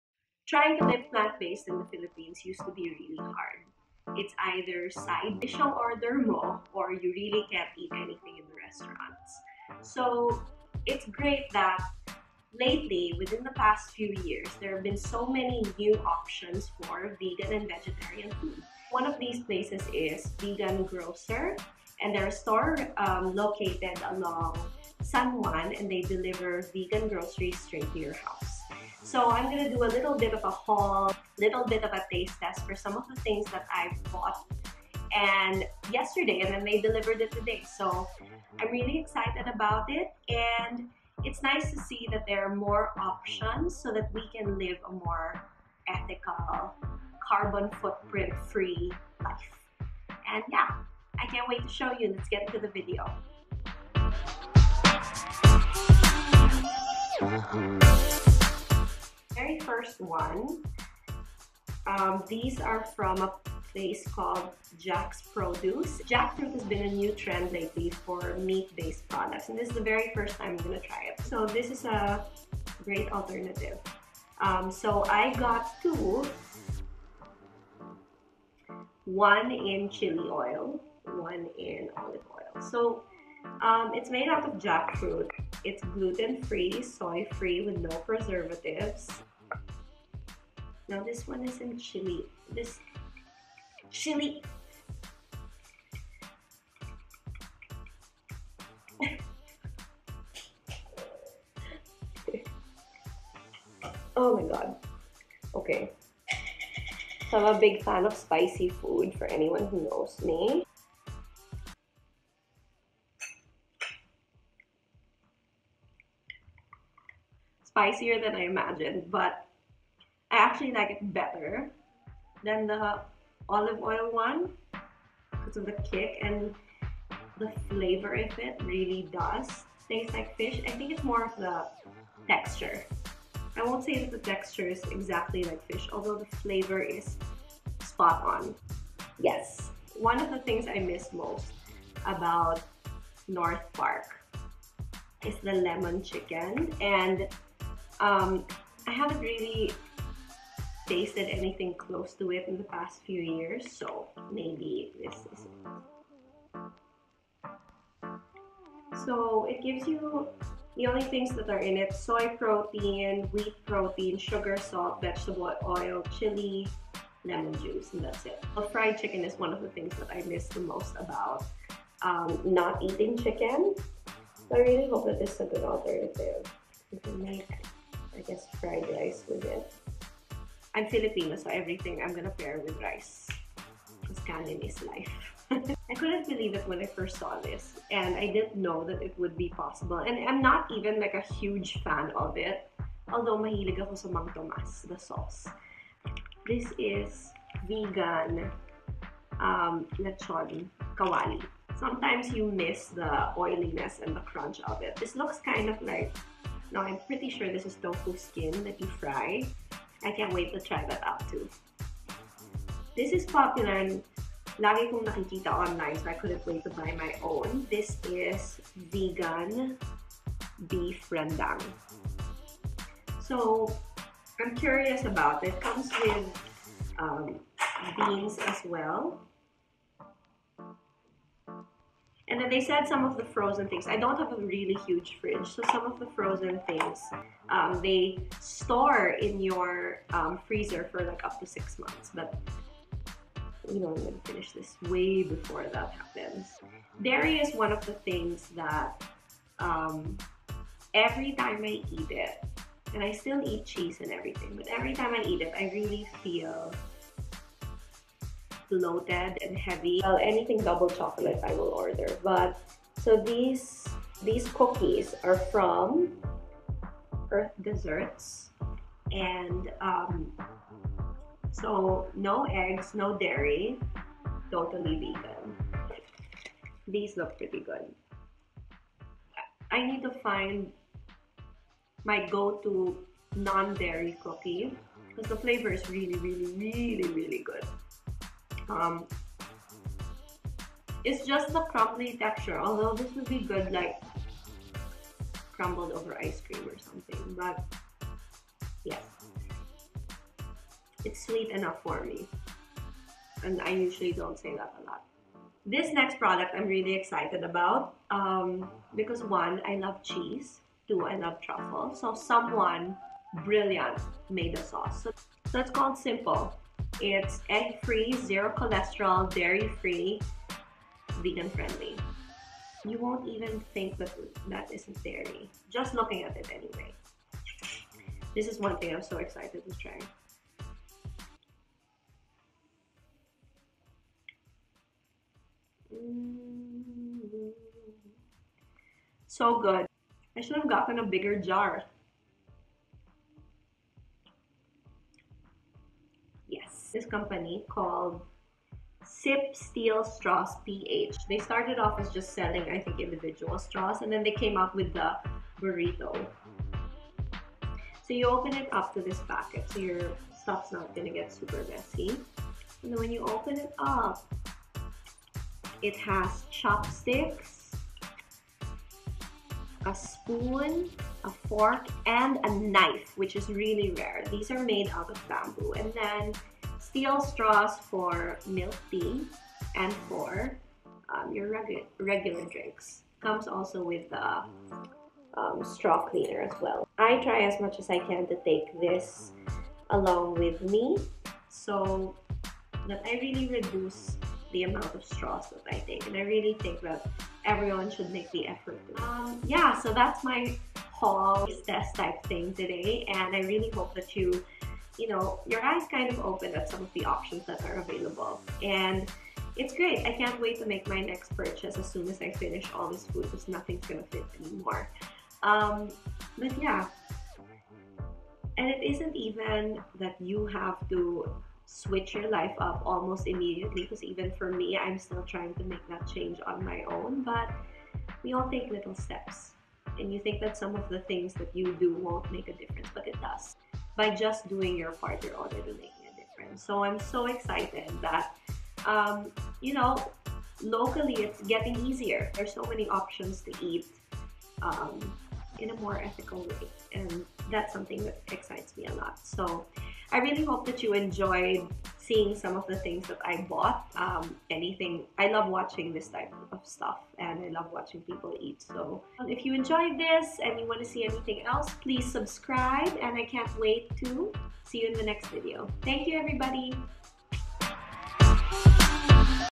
VEGAN FOOD HAUL (Philippines) || Rianna Yoga
Sharing with you guys some vegan finds!! Hopefully it'll show some of you guys that are looking towards a healthier lifestyle about the options you have out there! Also, ang sarap kumain!

Love, Rianna

Order from The Vegan Grocer!